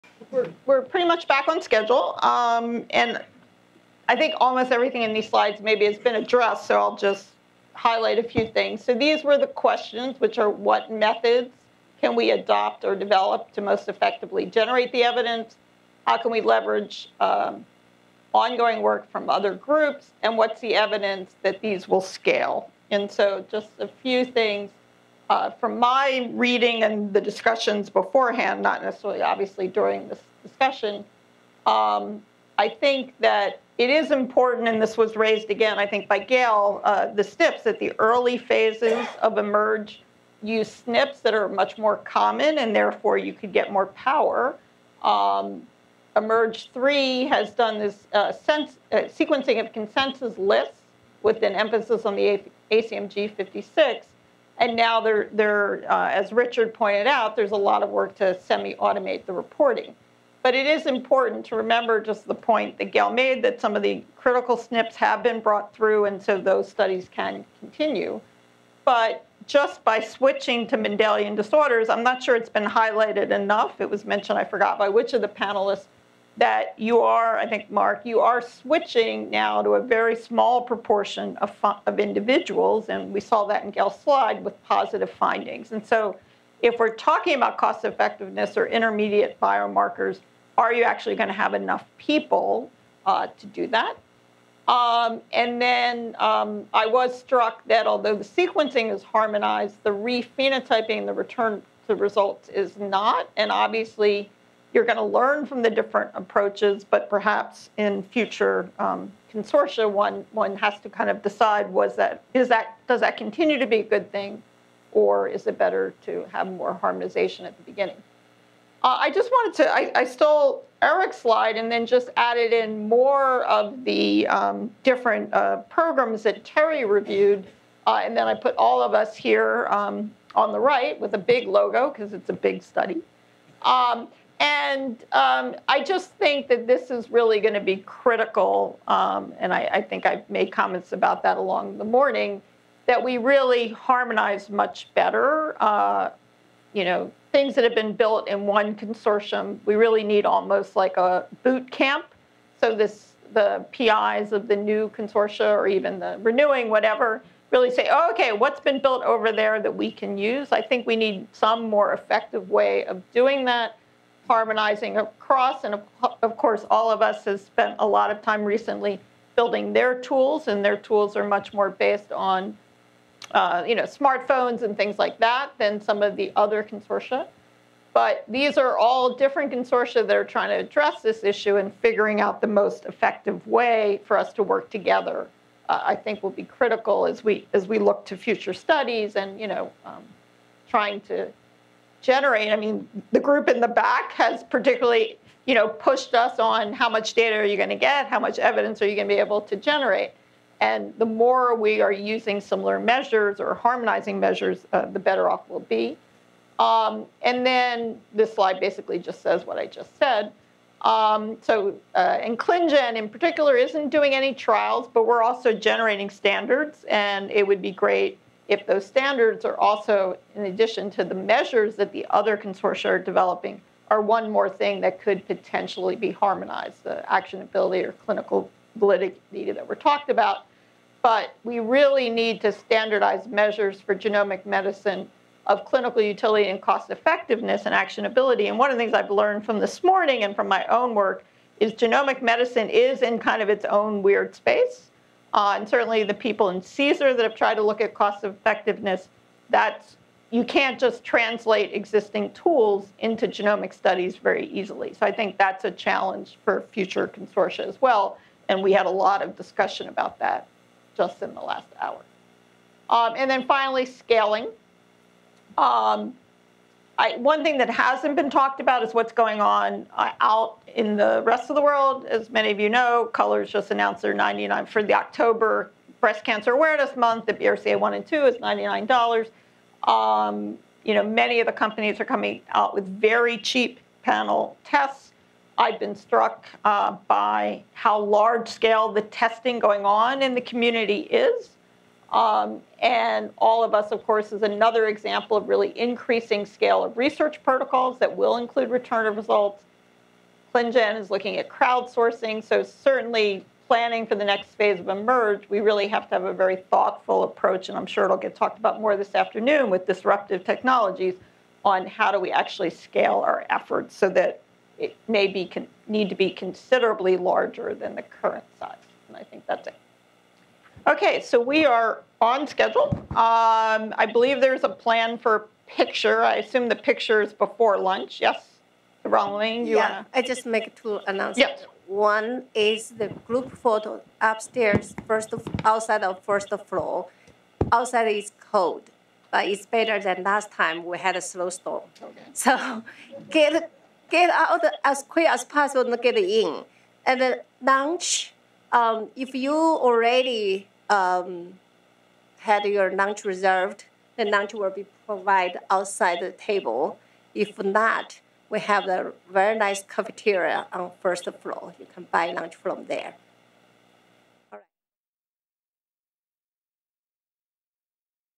Evidence Generation for Genomic Medicine (Summary) - Sharon Plon
October 30, 2017 - The National Human Genome Research Institute (NHGRI) hosted a program review workshop entitled eMERGE and Beyond: The Future of EHR and Genomics. The Electronic Medical Records and Genomics (eMERGE) network conducts genomic discovery and clinical implementation research by leveraging data from large biorepositories linked to electronic medical records. The workshop served to highlight accomplishments of eMERGE as well as identify potential eMERGE future directions. The workshop will identify gaps, discuss challenges and define future opportunities in genomic medicine research.